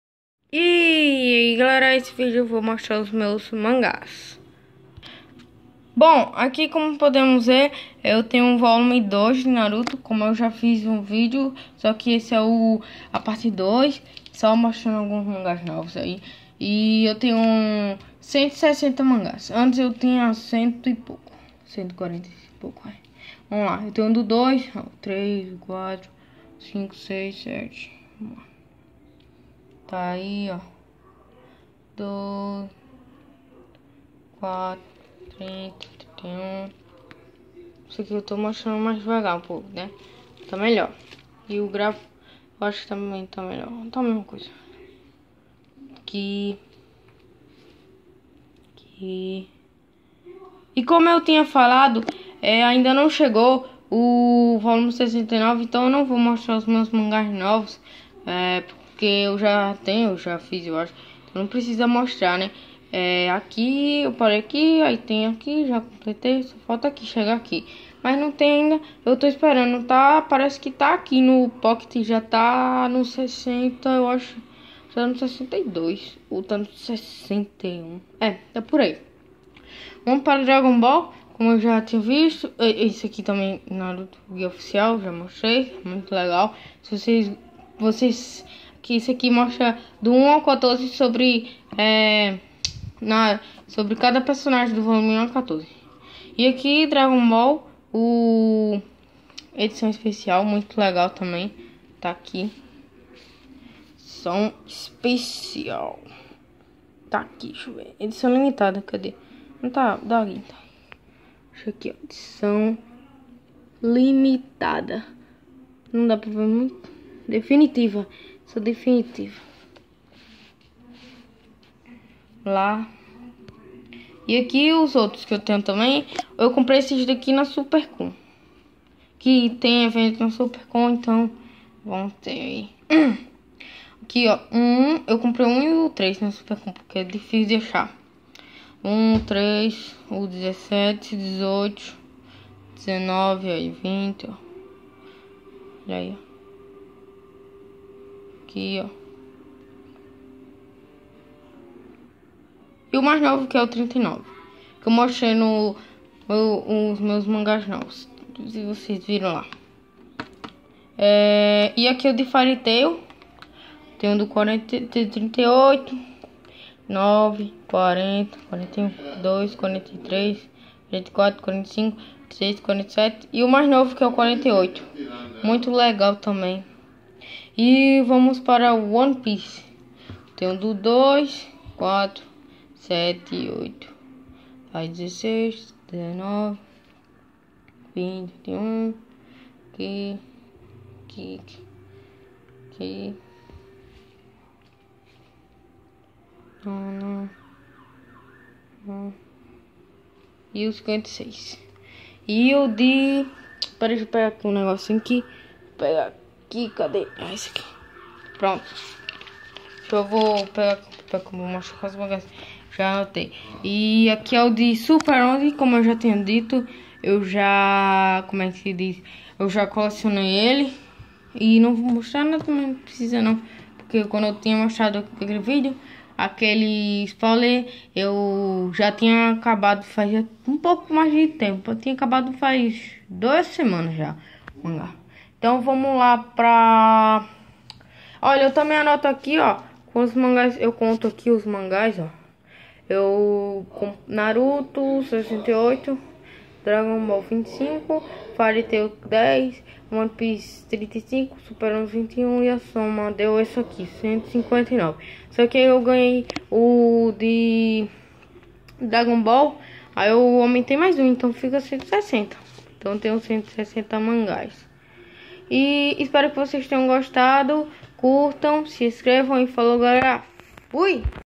Meus mangás - 160 mangás #2
68 mangás de naruto / 26 de dragon ball / 10 de fairy tail / 35 de one piece / 21 de super onze